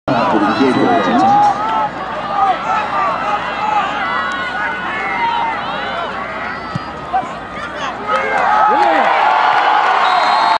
Columbia puts first 3 points on the board at Shen
Special teams open the 2013 season with a 41 yard field goal